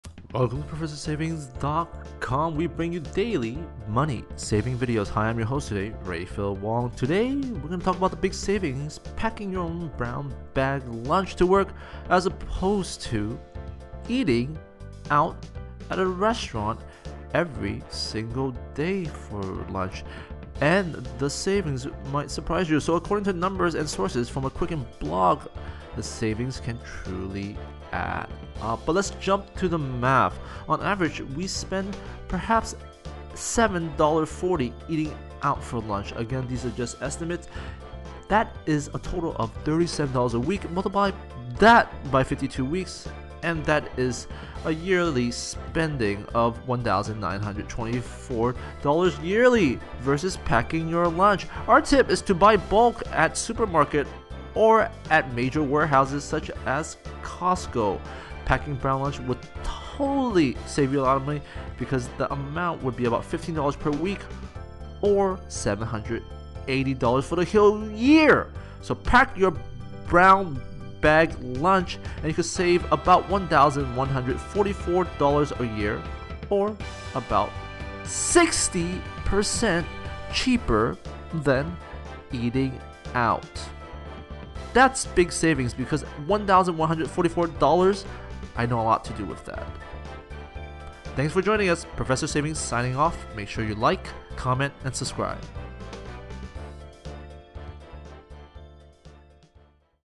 How much I can save bringing lunch to work- Professor Savings
Professorsavings teaches finance basics on mobile.

thanks

Professor Savings